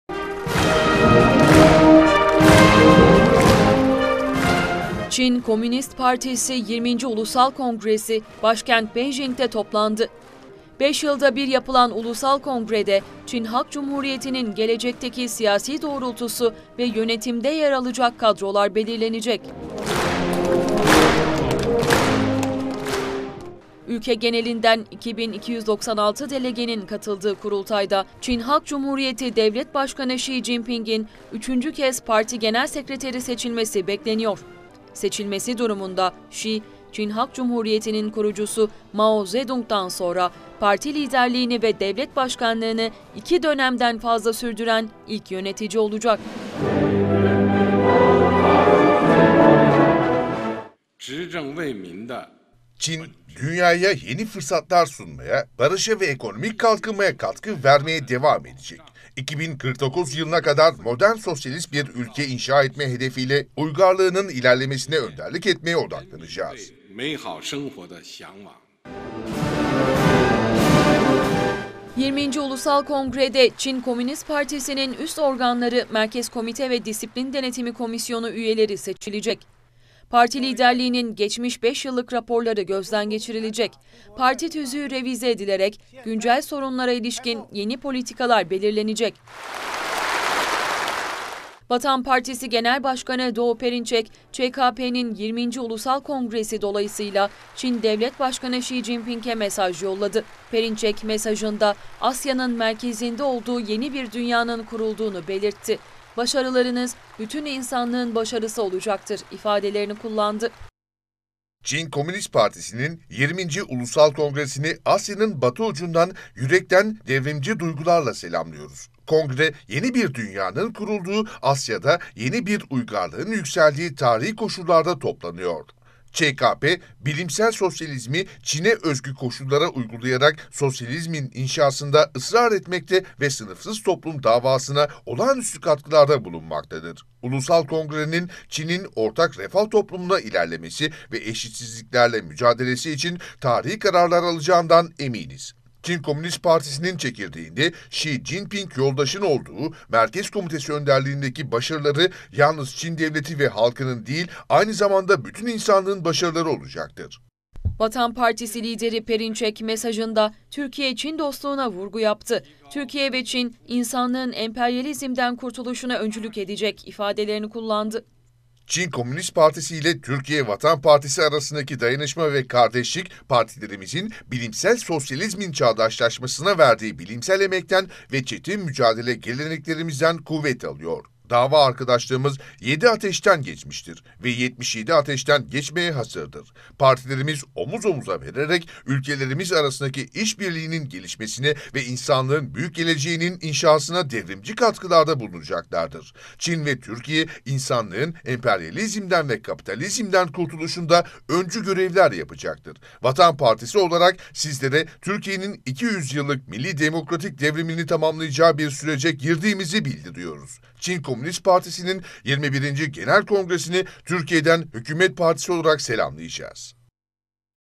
ÇKP 20. Kongresi toplandı: Şi Cinping kongreyi güvenlik vurgusuyla açtı
Çin Komünist Partisi’nin 20. Ulusal Kongresi, Başkent Beijing’te yapılıyor. Çin’in en önemli siyasi toplantısına,  ülke genelinden yaklaşık 2 bin 300 delege katılıyor. Kongrede, Çin Devlet Başkanı Şi Cinping’in 3. kez Parti Genel Sekreteri seçilmesi bekleniyor. Şi Cinping, kongrenin açılışında yaptı.